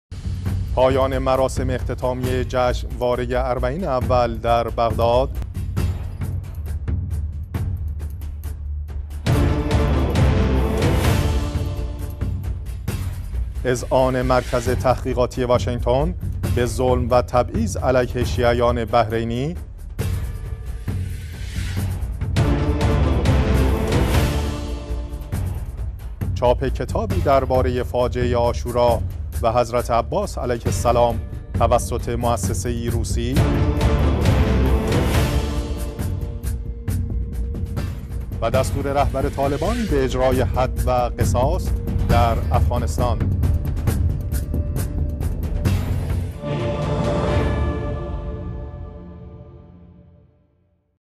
سرخط خبرها: سه شنبه 24 آبانماه 1401
۱-  پایان مراسم اختتامیه جشنواره "اربعین اول" در بغداد
۲-  اذعان مرکز تحقیقاتی واشنگتن به ظلم و تبعیض علیه شیعیان بحرینی
۳-  چاپ کتابی درباره فاجعه عاشورا و حضرت عباس علیه السلام توسط موسسه ای روسی
۴-  دستور رهبر طالبان به اجرای حد و قصاص در افغانستان
۵-  آیت الله العظمی شیرازی: پیوند عضو کافر به بدن مسلمان اگر پس از پیوند، جزء بدن مسلمان محسوب شود، پاک خواهد بود
۶-  سفر دو میلیون زائر برای ادای عمره به عربستان
7 - 170 هزار ایرانی طی ده سال اخیر در تصادفات جاده ای جان باخته اند